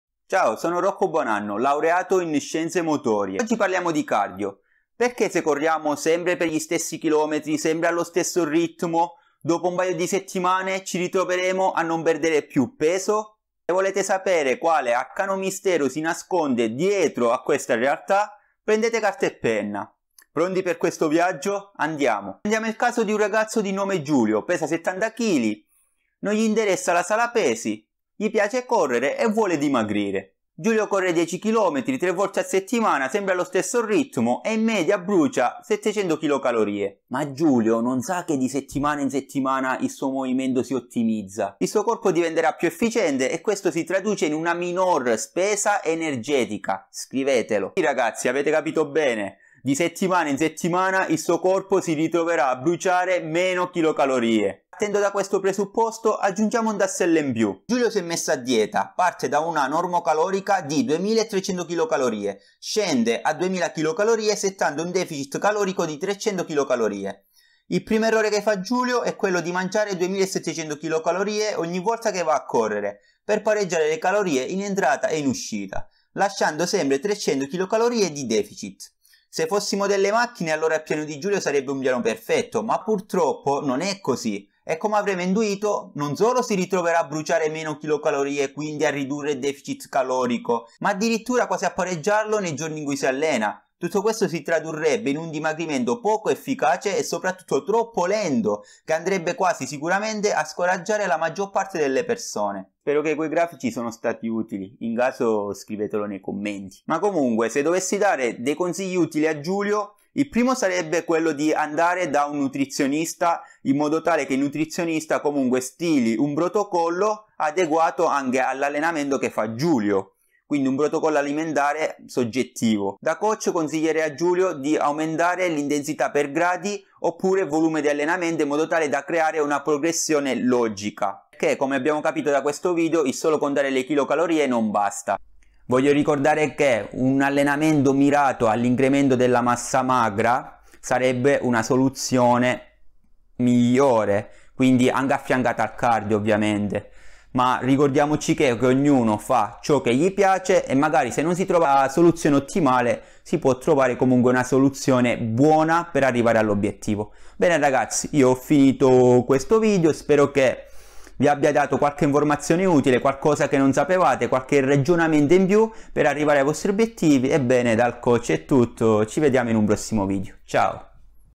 CARDIO QUELLO CHE NON SAPEVI!
Ciao ragazzi, sono Rocco Bonanno laureato in scienzemotorie
oggi parliamo di cardio! Sapevi che il cardio se fatto in questo modo può essere controproducente?